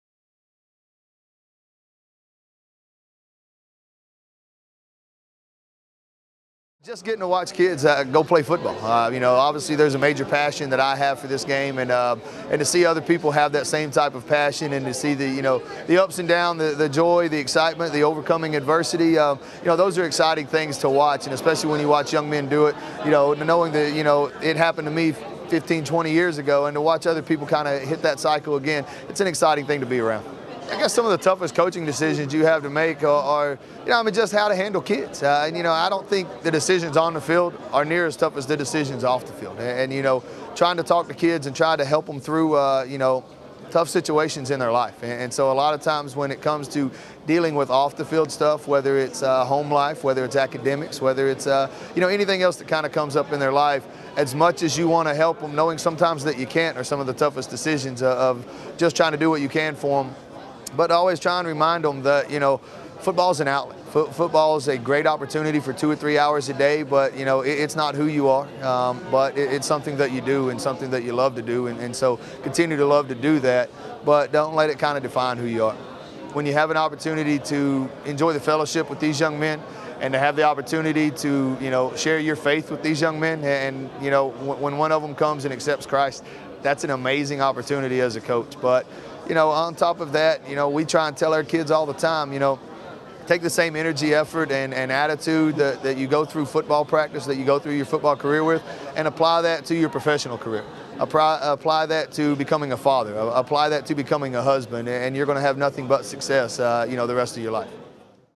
Powell High's Matt Lowe talks about what it means to be a head coach
Powell High School football coach Matt Lowe talks about what it means to be a head coach during Prep Media Day on July 15, 2011.